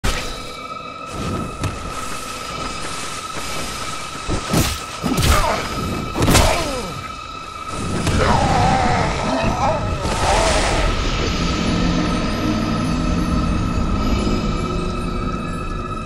Parry practice gone wrong
He called his friends :-(

And I'm shit at parrying with whips.